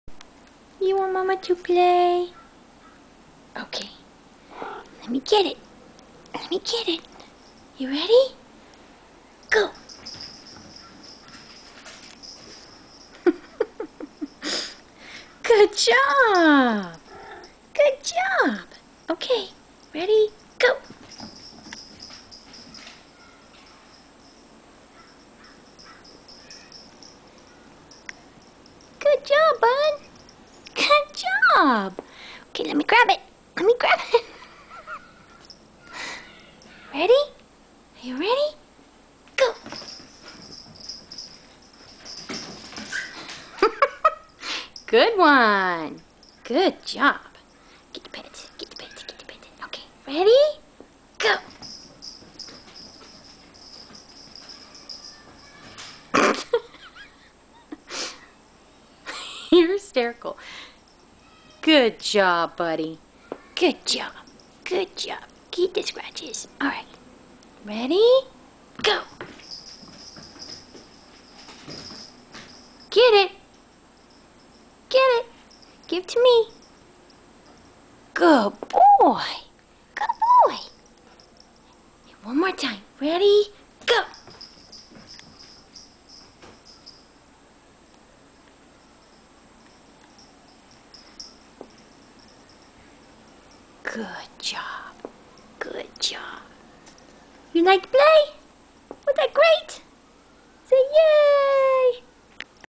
My Cat Mojo Plays Fetch
I swear he thinks he's a puppy! Me and my cat Mojo playing fetch...